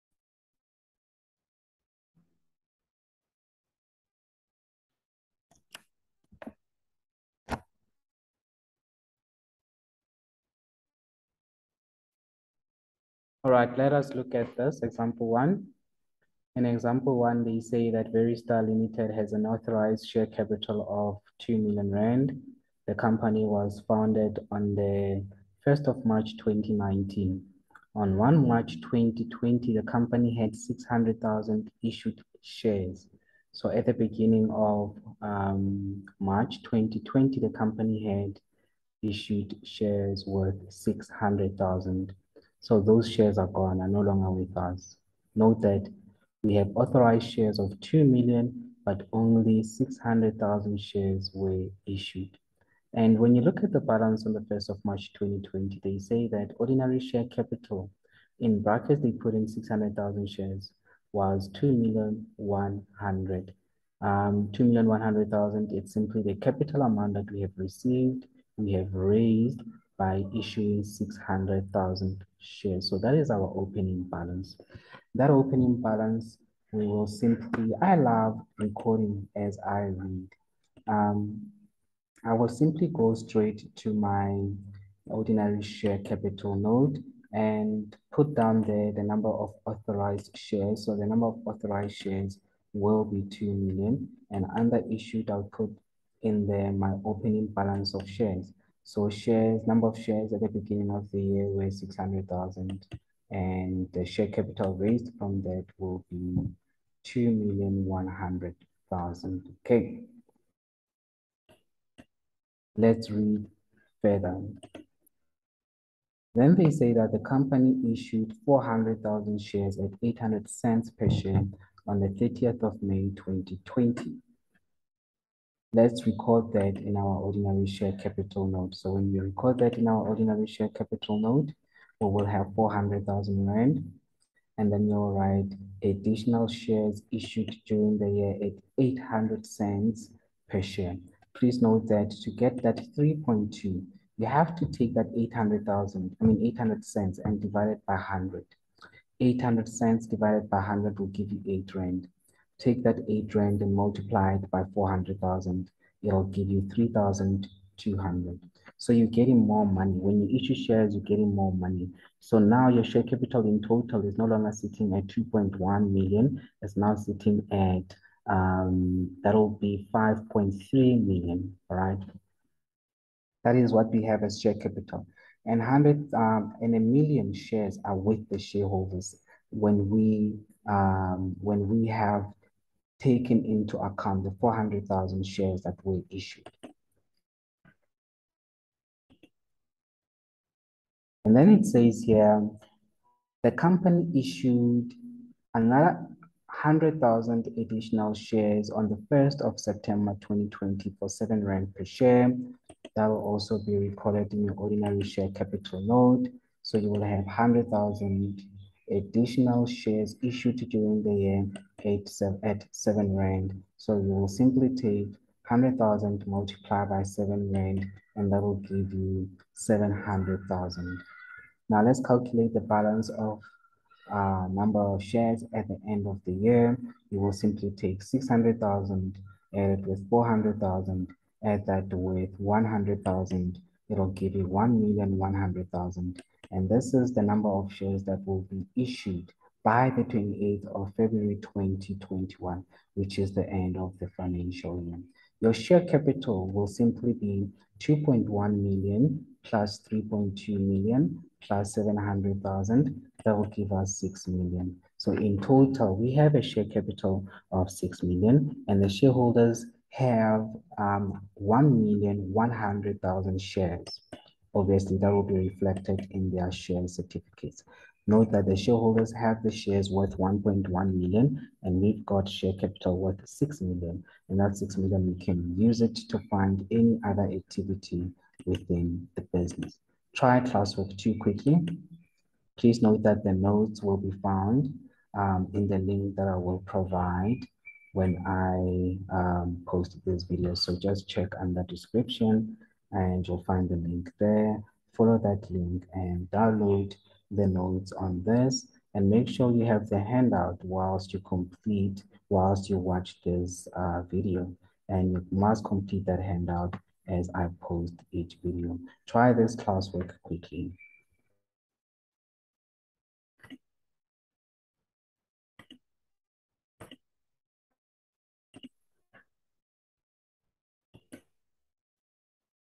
Financial accounting of companies; Ordinary Share Capital and Retained Income note - lesson 3
Veritas College Springs
Refiloa Litjamela on Facebook and LinkedIn